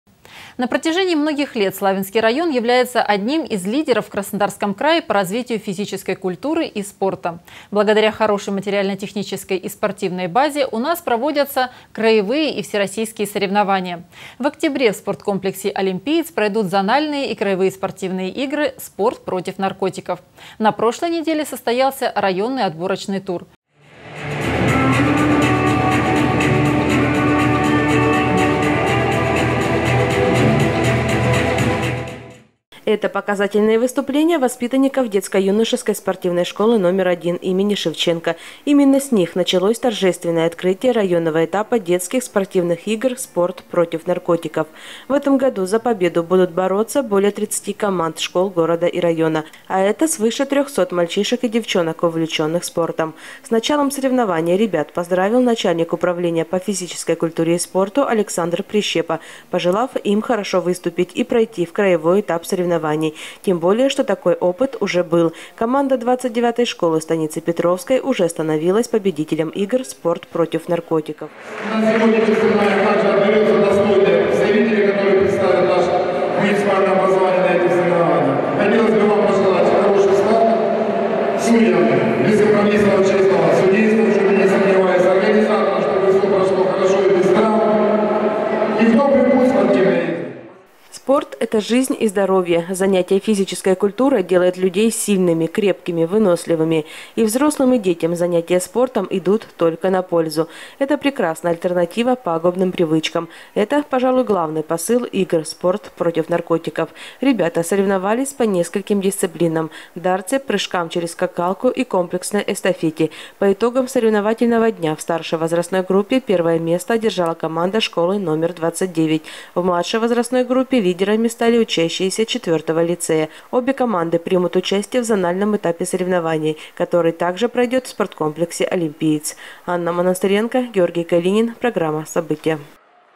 Спортивные игры «Спорт против наркотиков»
На протяжении многих лет Славянский район является одним из лидеров в Краснодарском крае по развитию физической культуры и спорта. Благодаря хорошей материально-технической и спортивной базе у нас проводятся краевые и всероссийские соревнования. В октябре в спорткомплексе «Олимпиец» пройдут зональные и краевые спортивные игры «Спорт против наркотиков».  На прошлой неделе состоялся районный отборочный тур